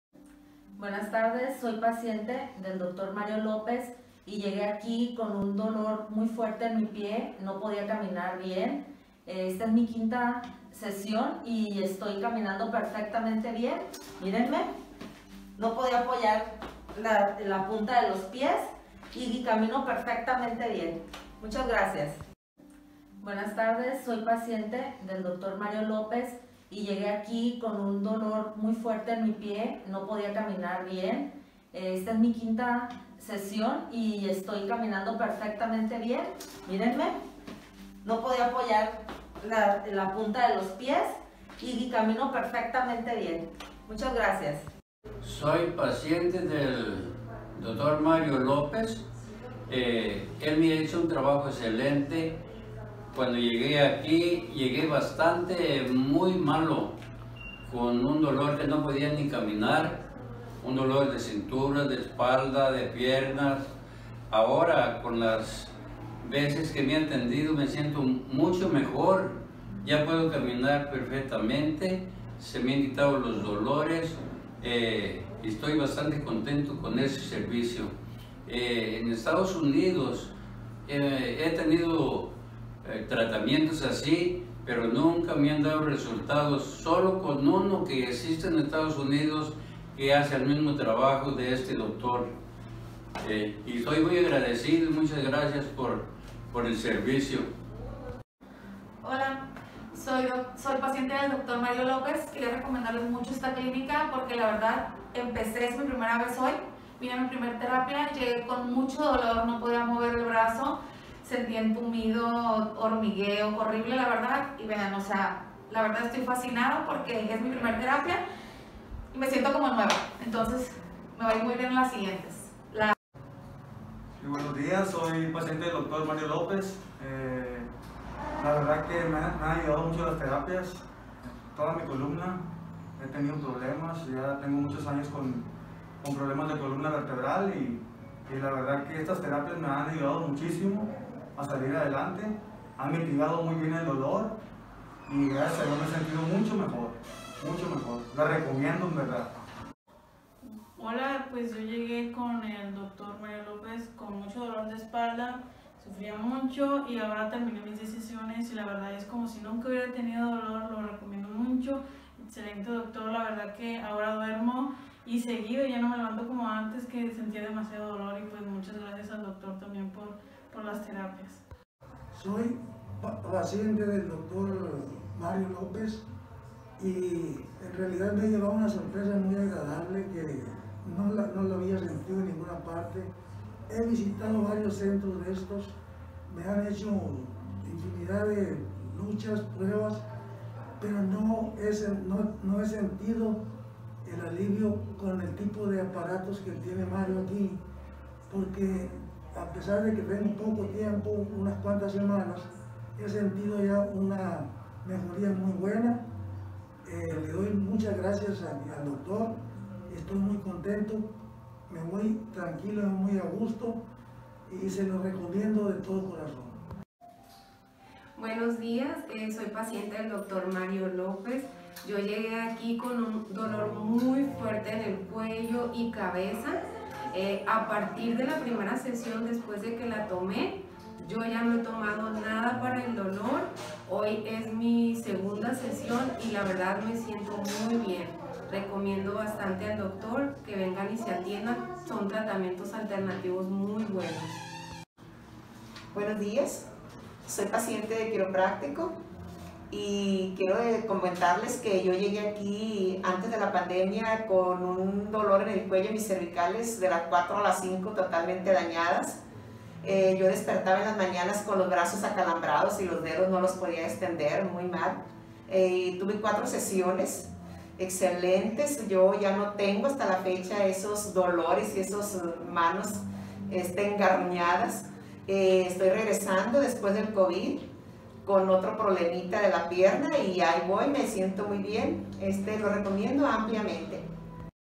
📣 Dolores de Cuello y Espalda RESUELTO | Testimonios de Pacientes Quiroprácticos
🌟 En este video, compartimos testimonios impactantes de pacientes que han encontrado solución a sus dolores de cuello y espalda. 👀 Playlist de Ajustes

Escucha cómo cada uno de ellos ha transformado su vida y mejorado su bienestar gracias a tratamientos personalizados y efectivos. Diagnóstico Resuelto: Dolores de cuello y espalda. 🌟 Historias de Alivio y Recuperación

🔧 Lo que encontrarás en este video:
Relatos inspiradores de personas que superaron el dolor crónico.
Detalles sobre los tratamientos quiroprácticos que marcaron la diferencia.
Consejos prácticos para quienes buscan alivio de sus molestias.